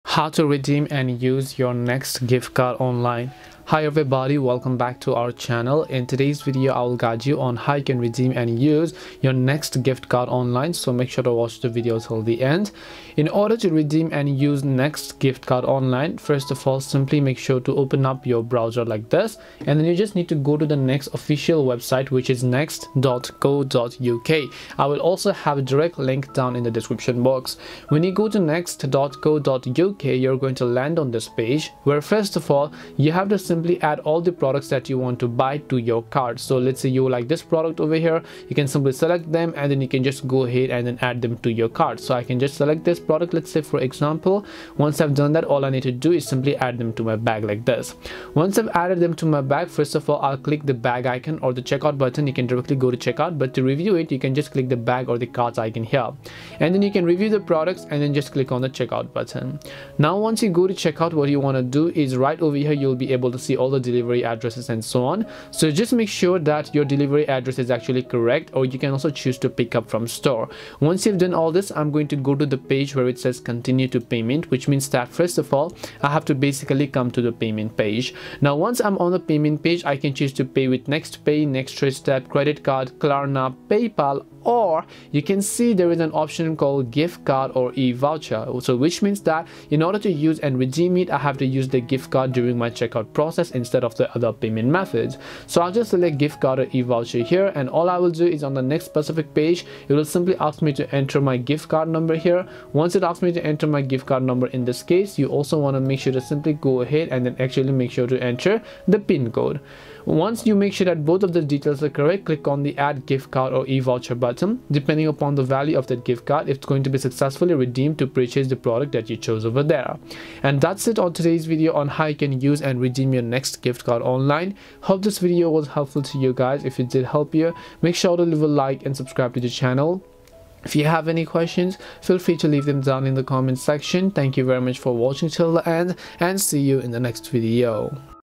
How To Use & Redeem Next Gift Card Online !
This video guides you in quick easy steps to use and redeem your Next Gift card online. So make sure to watch this video till the end.

~ Chapters:
0:00 Introduction
0:12 Redeem Next Gift Card Online
2:00 Conclusion